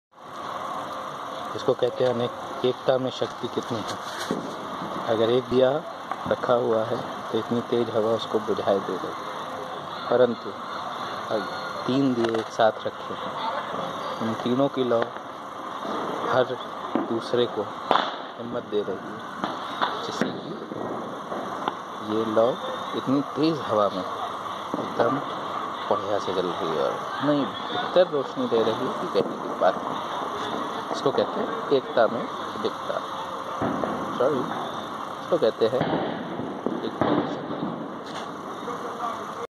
Unity makes the difference!!
When the wind blows, a single lighted diya can not last long however when the light of three different Diya are United, may the wind have the might but it won't be able to blow it off.

This best shows the power of Unity which is needed to succeed.
Embrace the power of Unity!
 Stay safe!!!